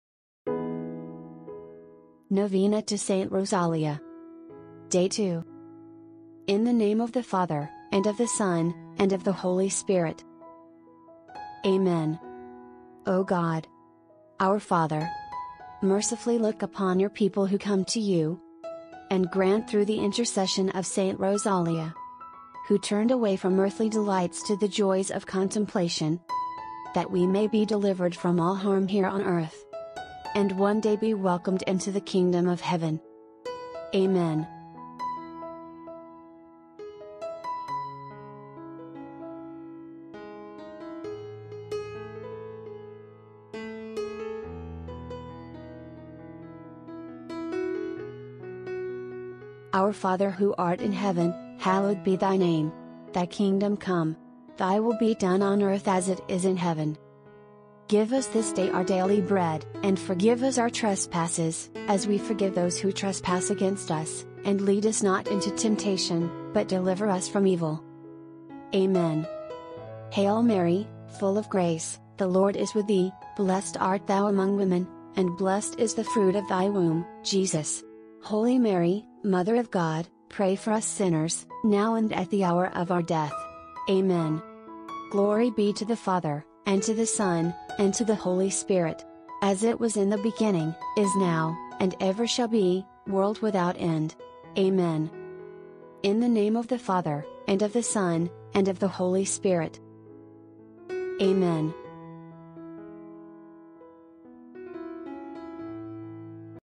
NOVENA TO ST ROSALIA | DAY 2
Novena Begins: August 26
Feast Day: September 4
Patron Saint of Palermo in Italy
O God, our Father, mercifully look upon Your people who come to You and grant through the intercession of St. Rosalia, who turned away from earthly delights to the joys of contemplation, that we may be delivered from all harm here on earth and one day be welcomed into the Kingdom of Heaven. Amen
Say 1: Our Father…
Say 1: Hail Mary…
Say 1: Glory Be…